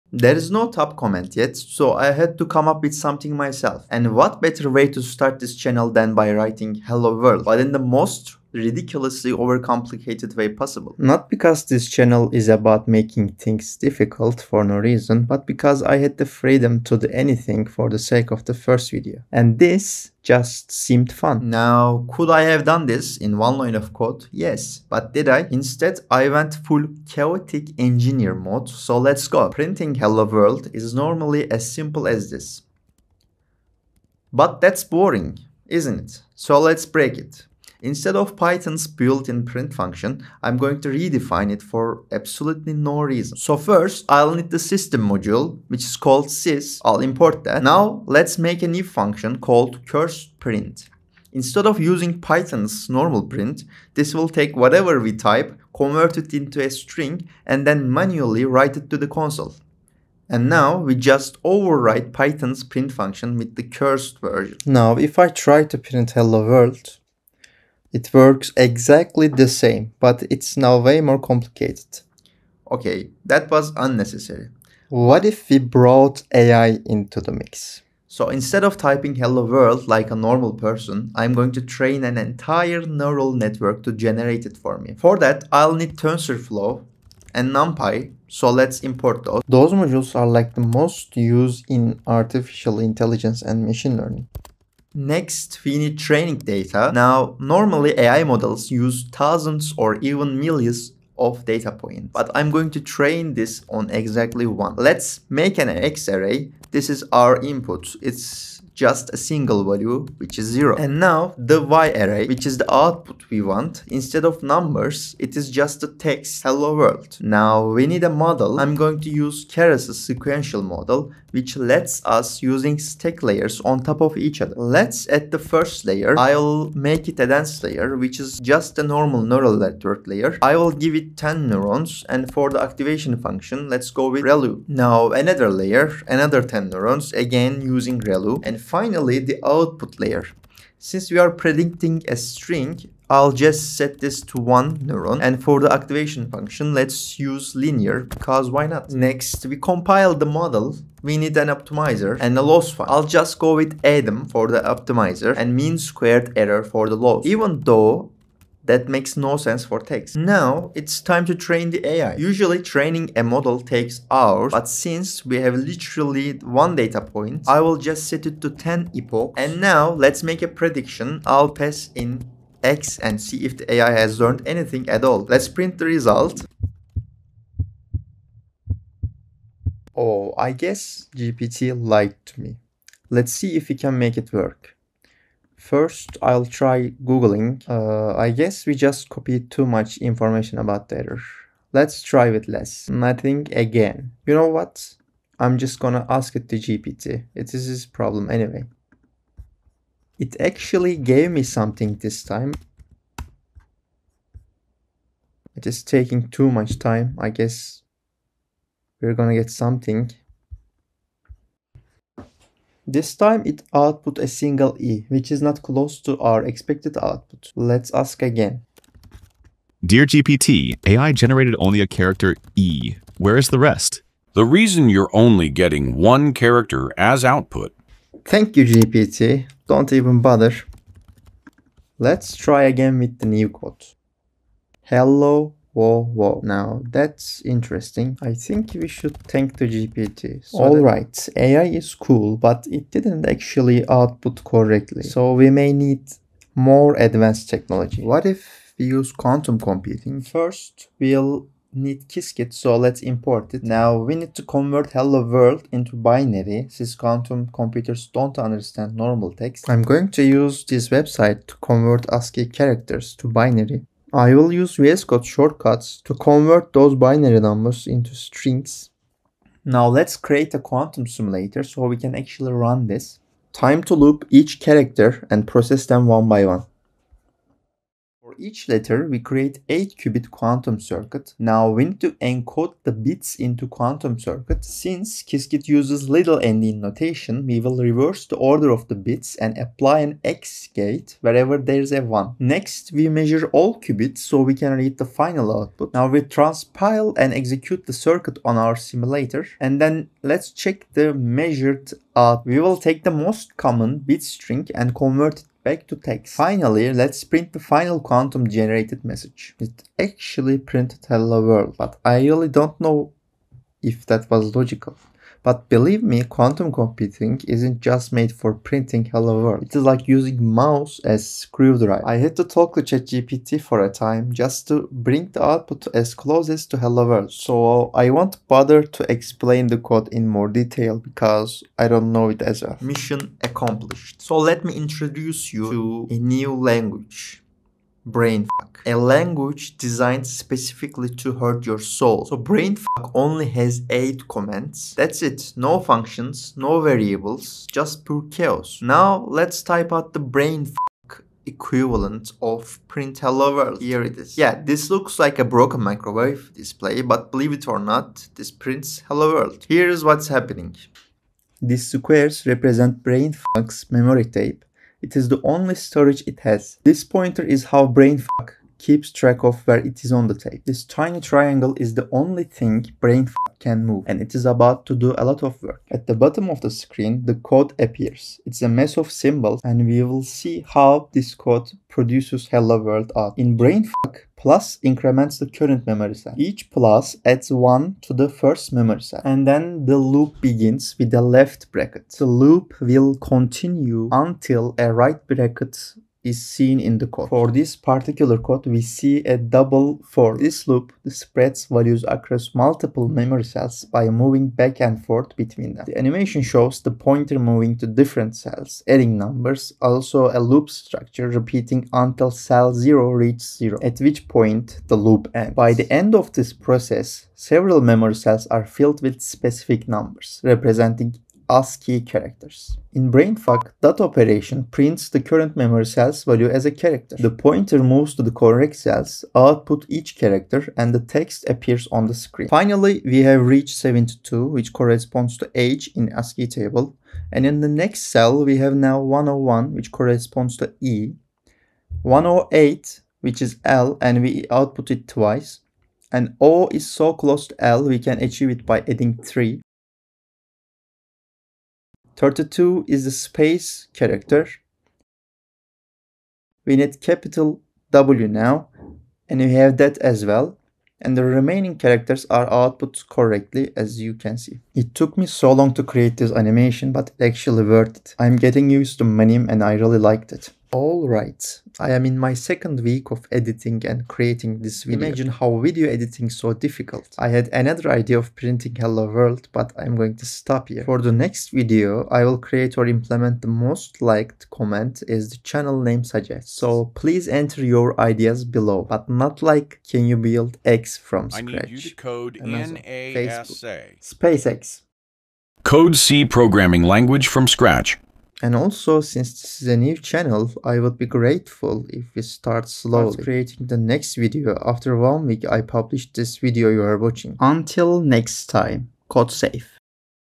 I will code whatever the top comment says - Hello Youtube
I am going to do whatever the top comment says in the next video.
Drop your ideas

00:00 Intro
00:41 Redefine print
01:14 Hello world with Neural Network
04:00 Hello world with Quantum Computing
05:33 Hello world with Brainfuck (animation)
07:52 Goodbye